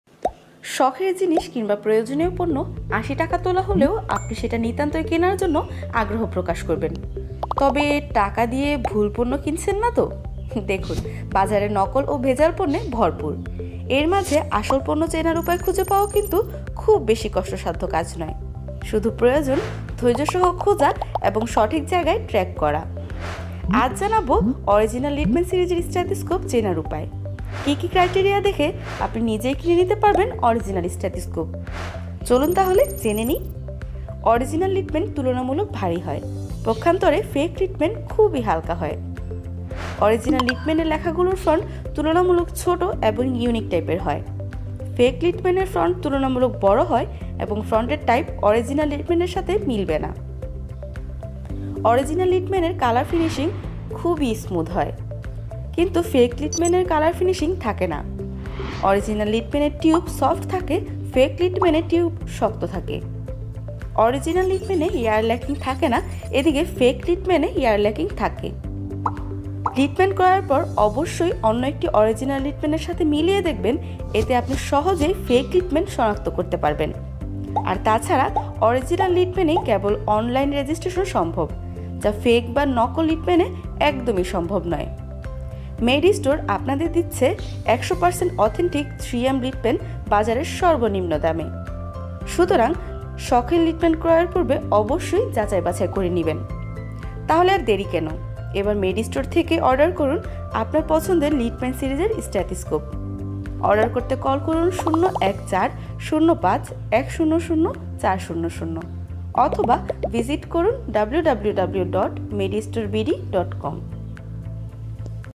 Product Explainer Video || FB Ads || Motion Graphics Video
Client: MedistoreBd
created This awesome video for product explain and promotion.